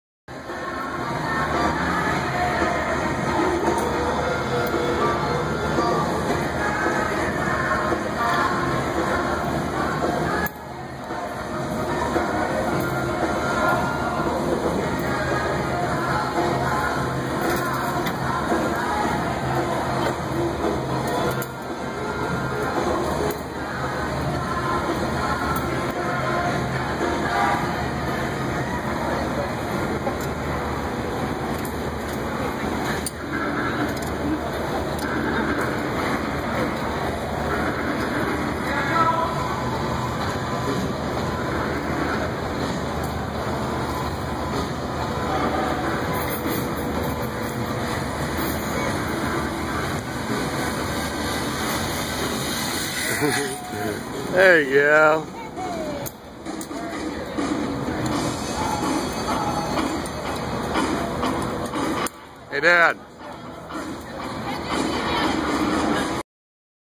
My dad zip-lining on our cruise ship.